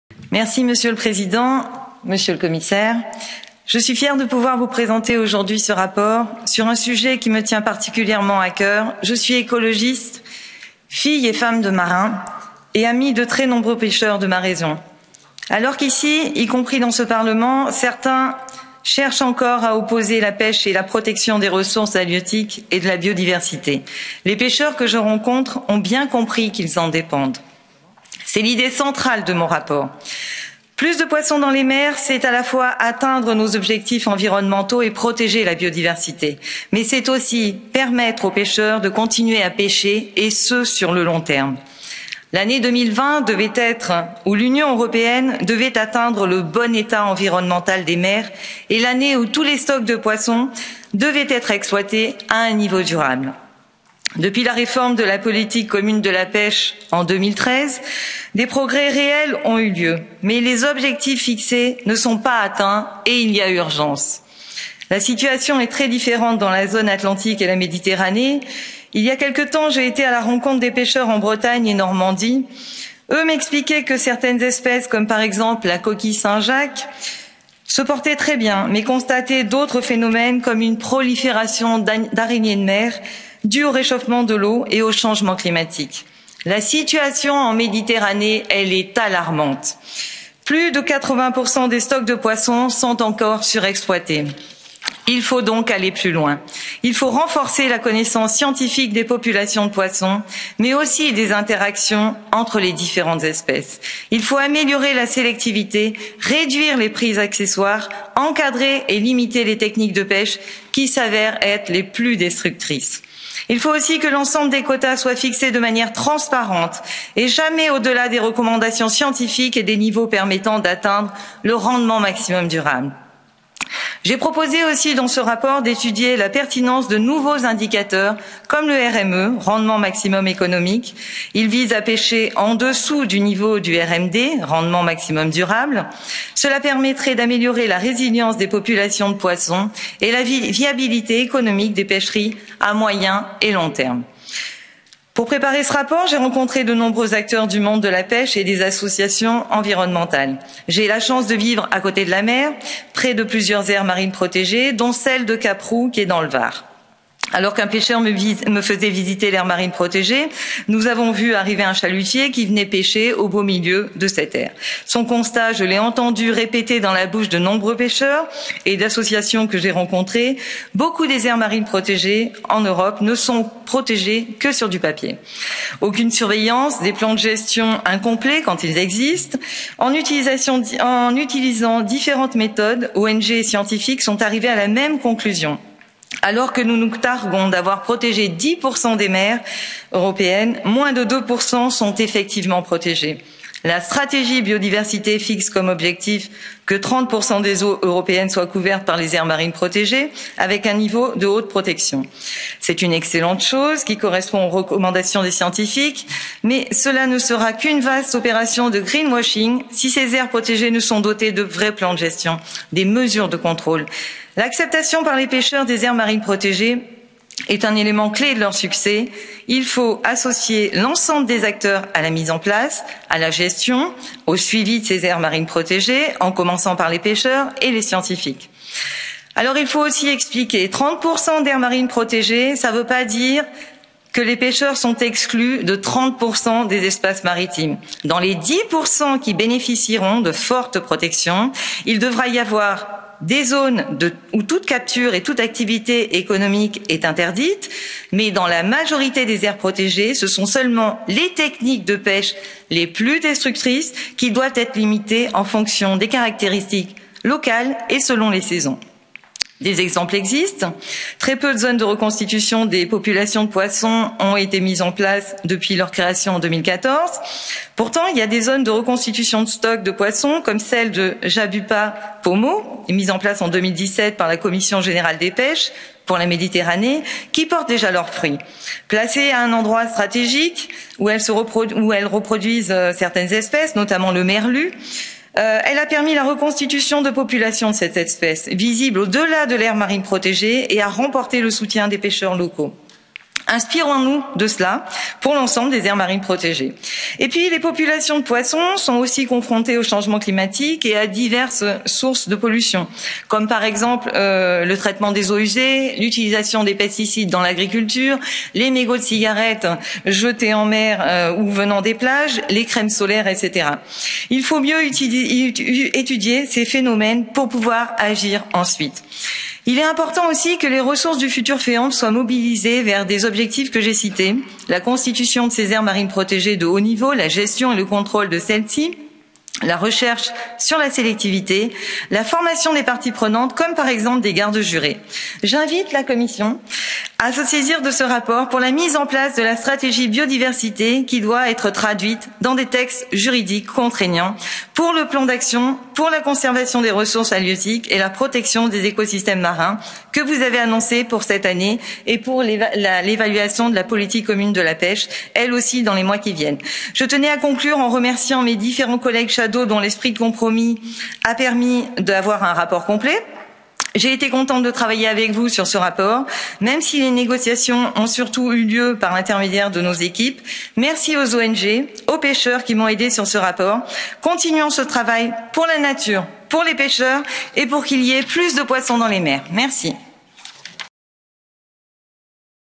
Caroline Roose sur son rapport "Plus de poissons dans les mers"
Le lundi 18 janvier, Caroline Roose a présenté son rapport "Plus de poissons dans les mers". "Plus de poissons dans les mers, c’est, à la fois, atteindre nos objectifs environnementaux et protéger la biodiversité et permettre aux pêcheurs de continuer à pêcher."